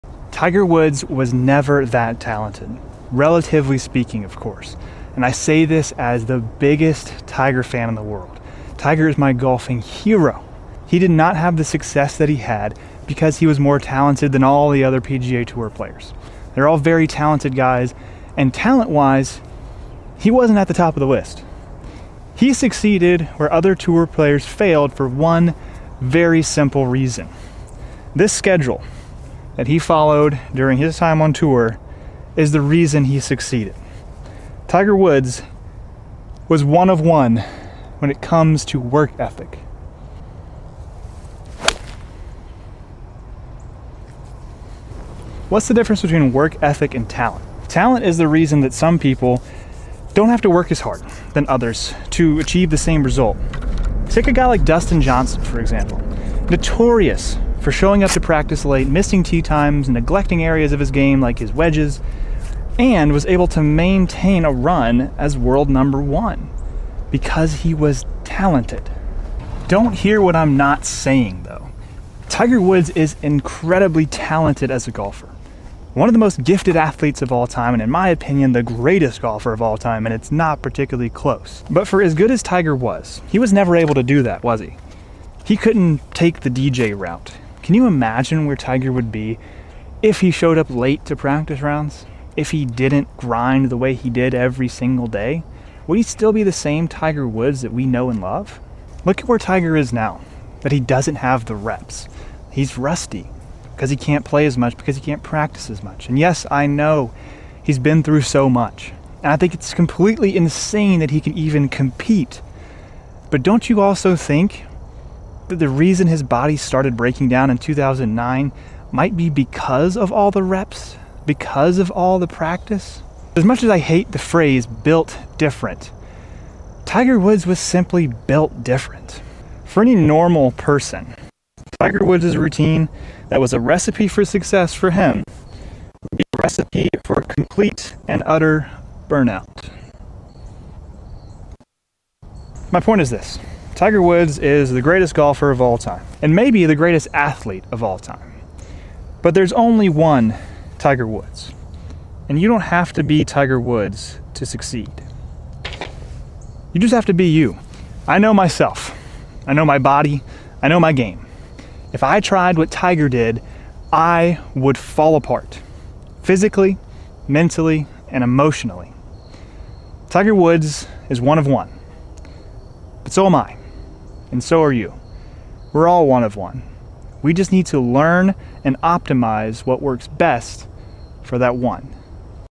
Why Tiger Woods Isn’t the Most Talented (But Is Still The GOAT)
It's rumored that Tiger Woods once told John Daly "If I had your talent I would have already passed 18 majors". Tiger is the greatest golfer of all time, and it's not close in my opinion, but he didn't win with talent, he won with hard work and discipline. What Tiger did isn't necessary for everyone, Tiger did what he had to do to succeed, but not everyone has to work that hard. We all have to find what works best for us as individuals.